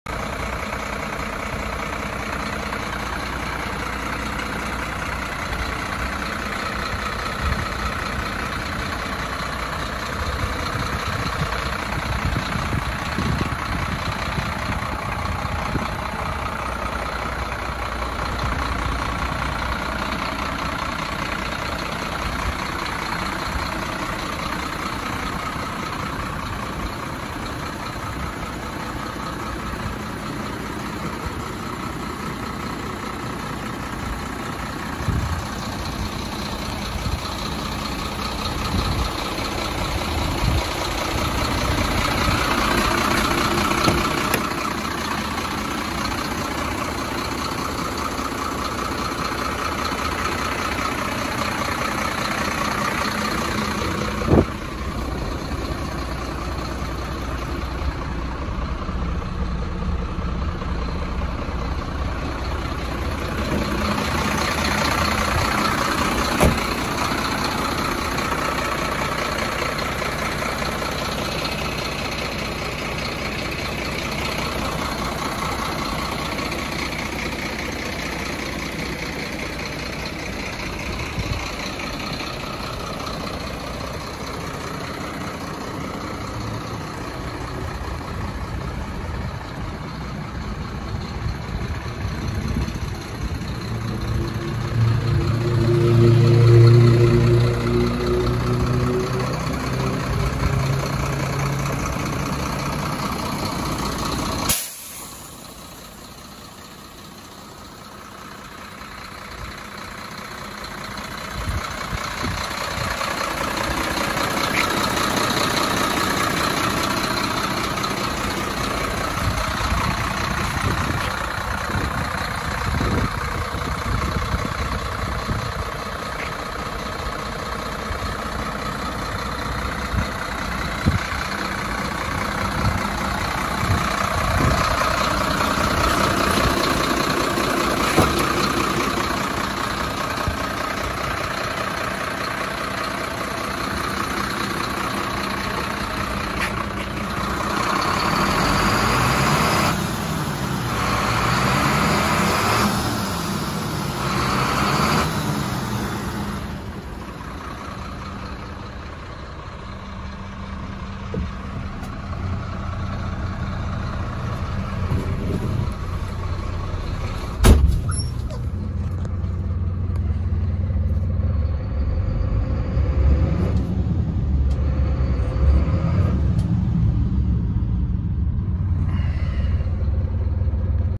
**SOLD** 2008 Mercedes Axor 2633 6x4 Cement Mixer / Hub Reduction - Dixon Commercial Exports Ltd
2008/57 Mercedes-Benz Axor 2633 Day Cab, 6x4, 8 Speed Manual Gearbox, Hymix Mix Cement Body / Equipment, In Cab Controls, Hub Reduction, Spring Suspension, Diff Lock, Engine Brake, Sun Visor, Roof Beacon, Vertical Stack Exhaust, Fully Working Order, 233,507 KM Only, Overall Great Condition.